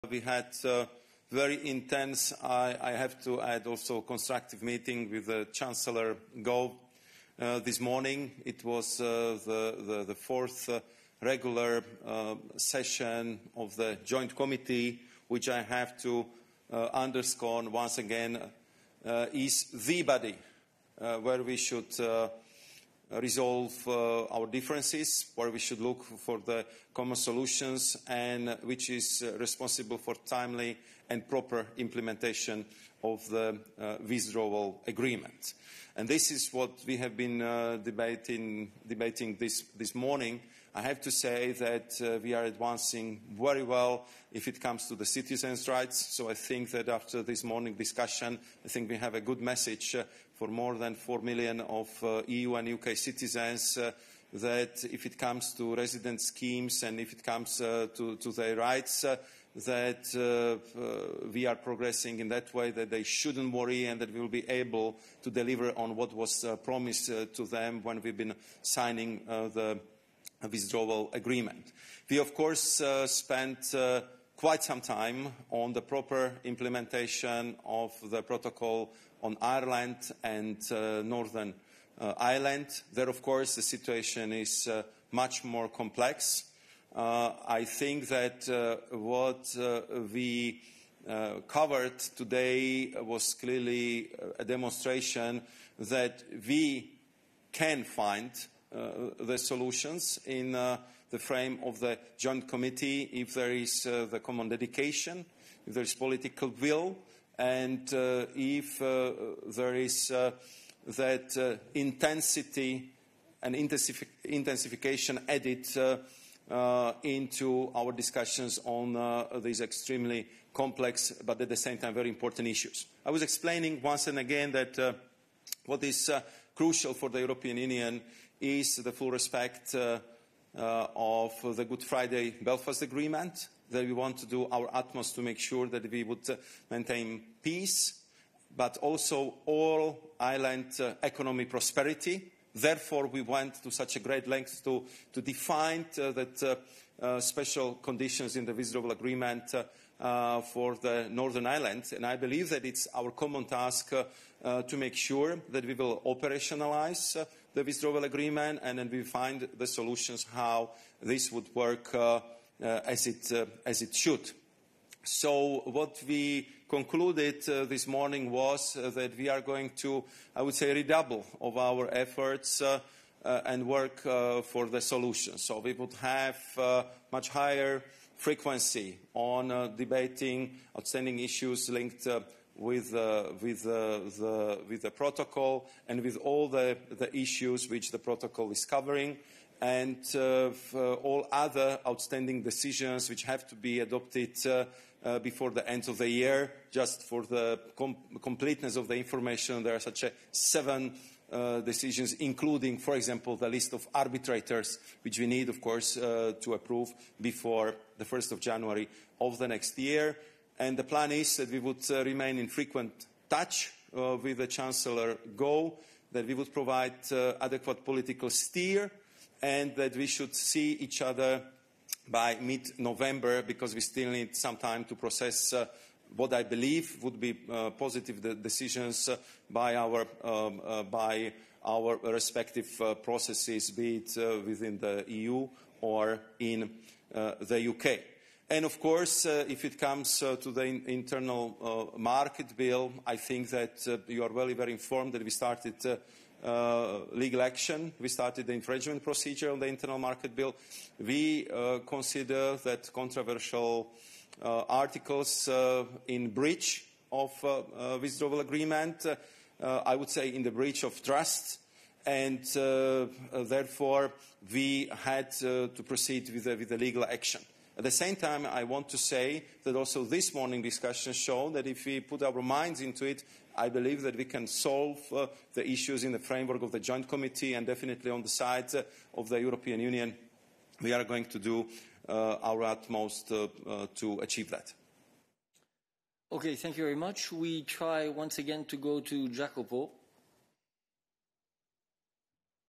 Brexit trade talks: EU says had 'intense and constructive' meeting with UK on withdrawal pact
The European Union and Britain today held intense and constructive talks on implementing the Withdrawal Agreement on the UK’s exit from the bloc that showed solutions are there “if we put our minds to it,” a senior European Commissioner said.

“We had a very intense... constructive meeting,” European Commission Vice President Maros Sefcovic told a news conference after a meeting earlier with Britain’s Minister for the Cabinet Office, Michael Gove, in the so-called EU-UK Joint Committee.

He said the two sides, at odds over a trade agreement and other aspects of their future relationship, would redouble their efforts to work for a solution ahead of the end of Britain’s standstill transition period on Dec. 31.

“What we concluded this morning was that we are going to ... redouble our efforts and work for the solution,” Sefcovic said.

“So we will have much higher frequency on debating outstanding issues linked with the (Northern Ireland) protocol, and all the issues which the protocol is covering, and all other outstanding decisions which have to be adopted before the end of the year.”

He said parts of London’s draft Internal Market Bill were in violation of UK’s EU divorce treaty.

“I believe we can solve the issues in the framework of the Joint Committee and definitely on the side of the European Union, we are going to do our utmost to achieve that.”